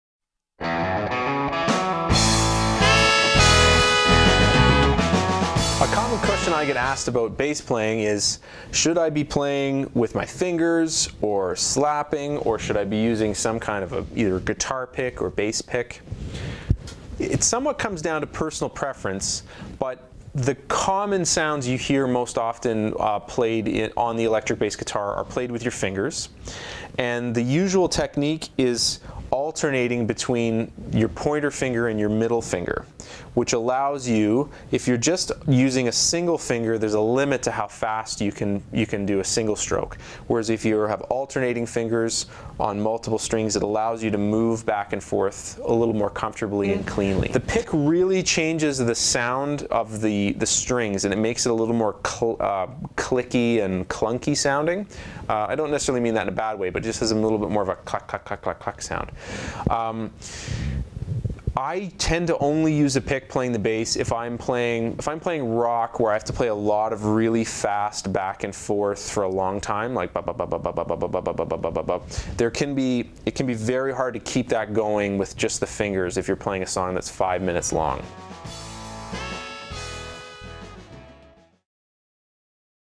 Bass Picks - Beginners Guide - The Music Coach
presents: Bass Picks - Beginners Guide

In this video tutorial, I'll show you what the different types of picks you can use on your bass guitar.